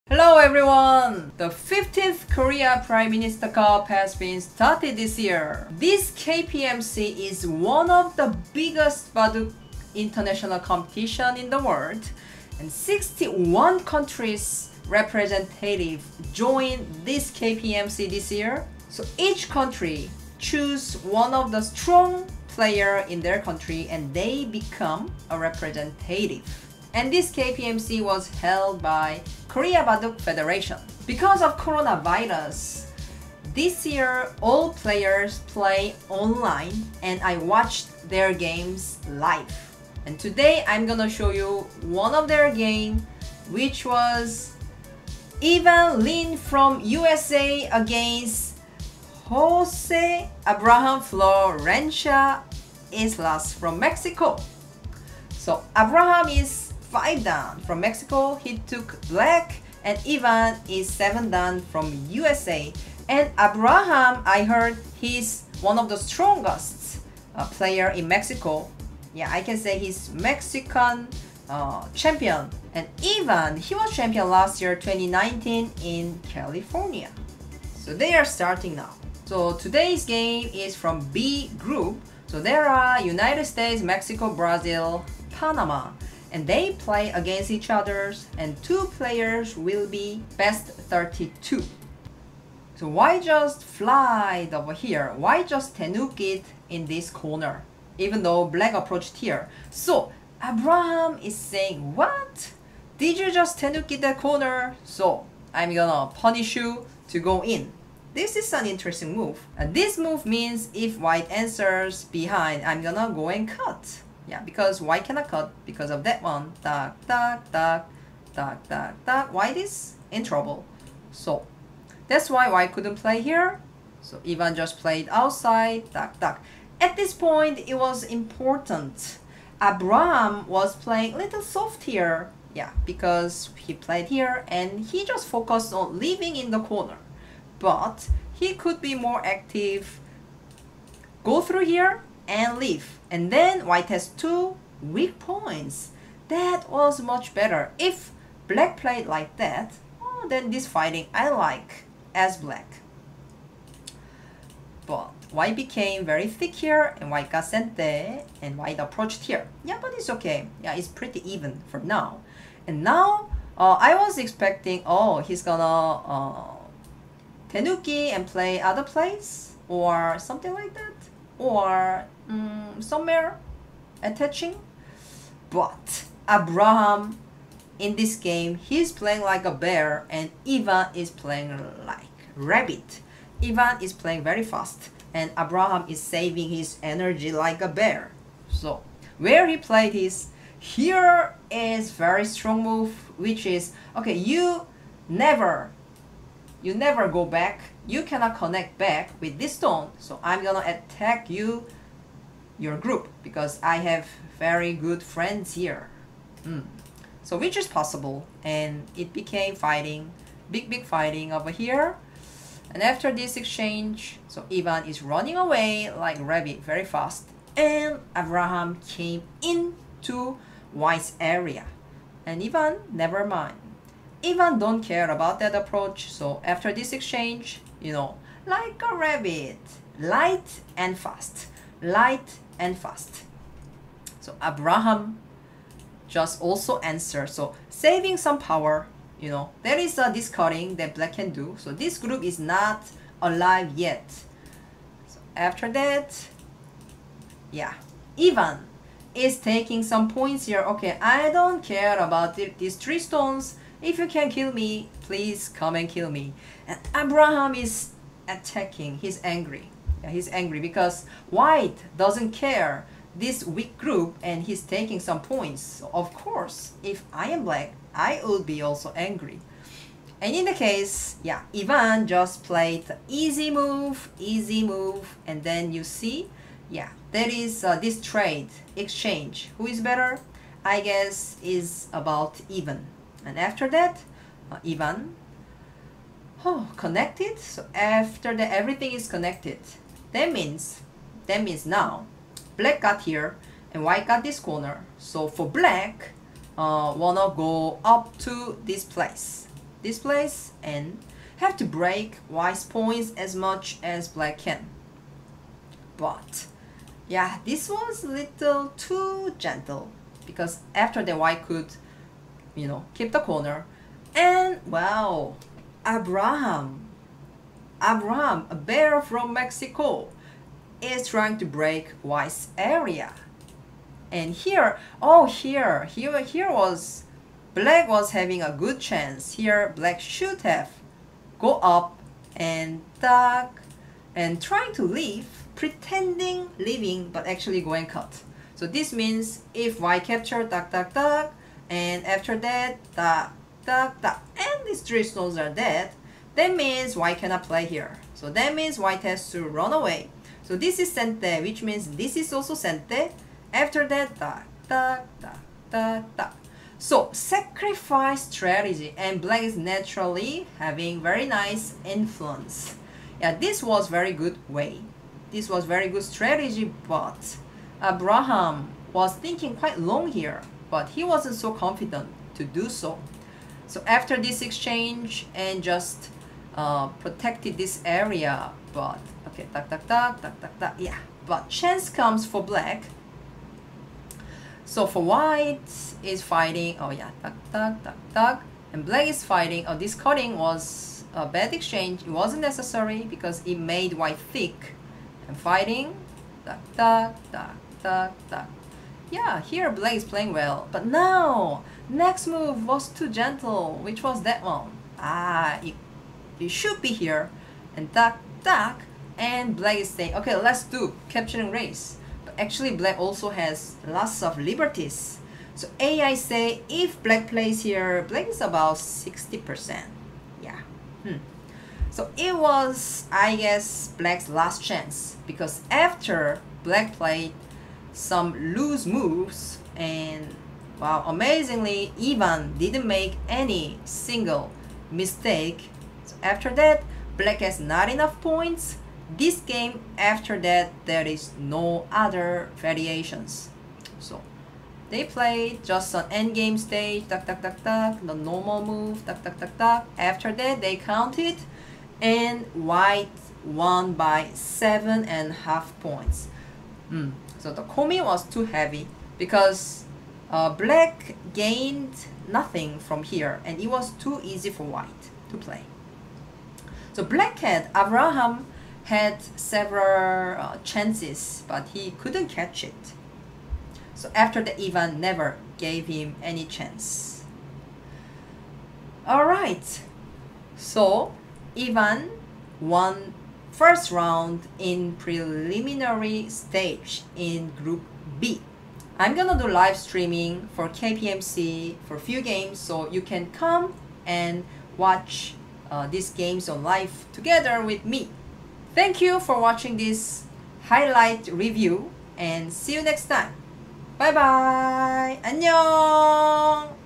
[KPMC] Evan Lin U.S.A vs Jose Abrahan Mexico Highlight reviewㅣGoproYeonwoo
The 15th Korea prime minister cup has been started!
Today's highlight review game is between Evan Lin 7D from U.S.A vs Jose abrahan from Mexico 5D
Enjoy the review :D